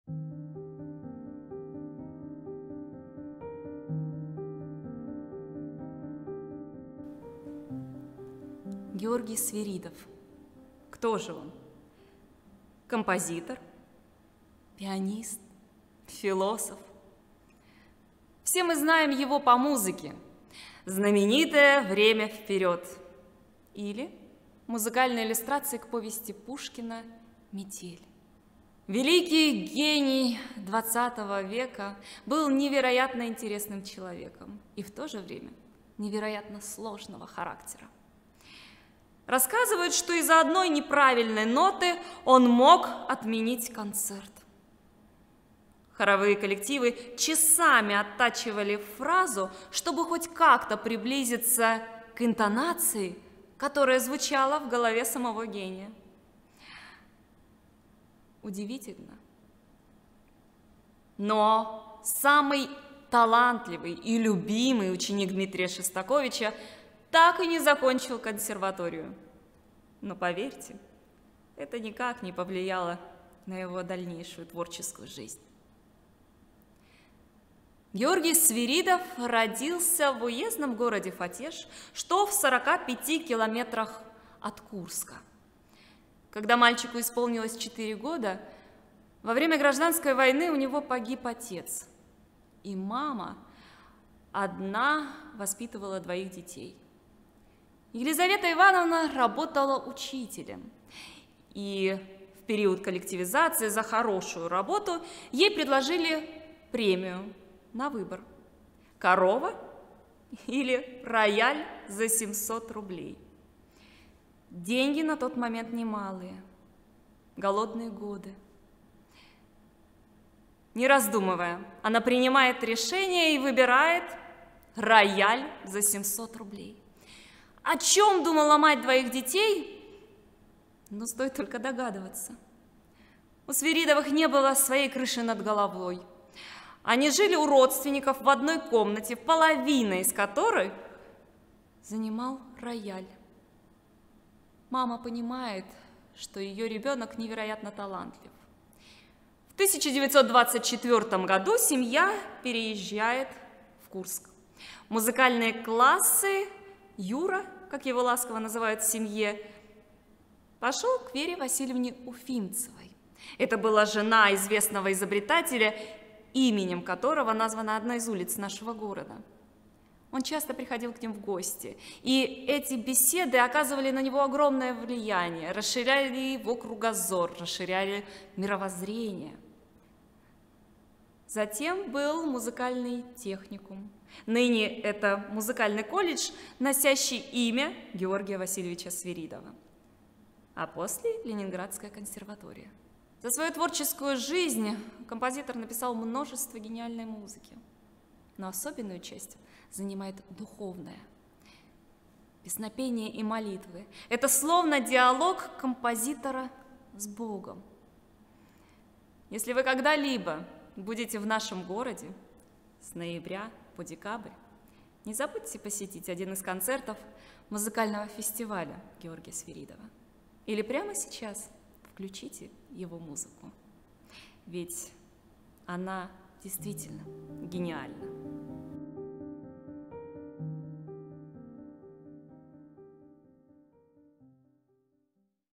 Музыкальный лекторий - Немного о Свиридове
Музыковед - Екатерина Демидова

Курская государственная филармония